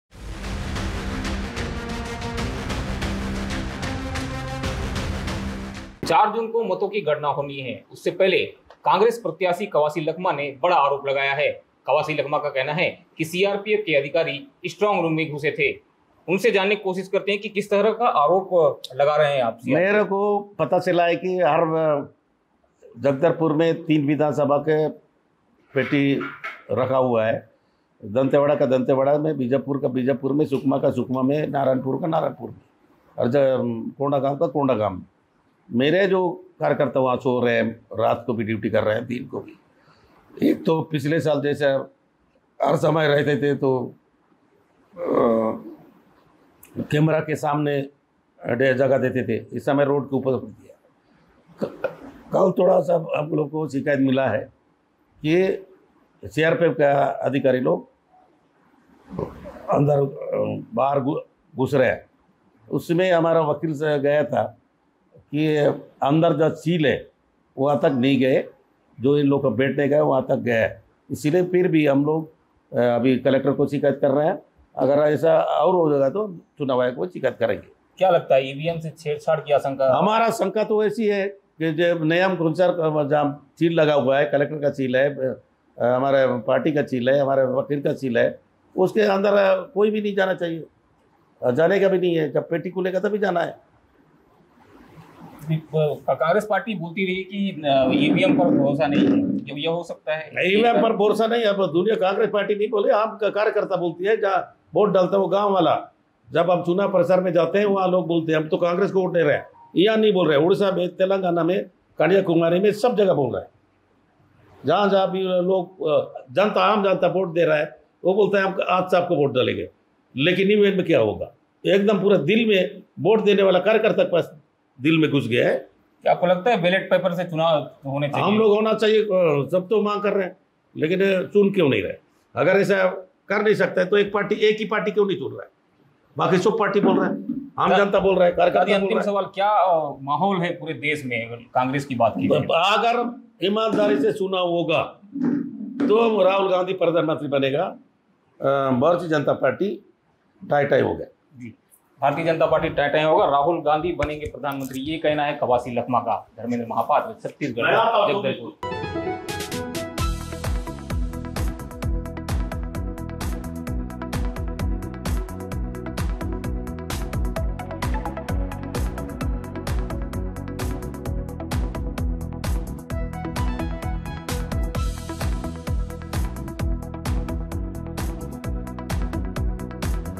Kawasi Lakhma Exclusive: बस्तर के नतीजों से पहले Lakhma ने कर दिया बड़ा खुलासा, हो जाएंगे हैरान!
Kawasi Lakhma Exclusive: बस्तर लोकसभा से कांग्रेस के प्रत्यासी और पूर्व मंत्री कवासी लखमा ने छत्तीसगढ़ Tak से खास चर्चा की. इस दौरान लोकसभा चुनाव के नतीजों से पहले लखमा ने कई खुलासे कर दिए, सुनकर रह दंग रह जाएंगे. देखें खास चर्चा...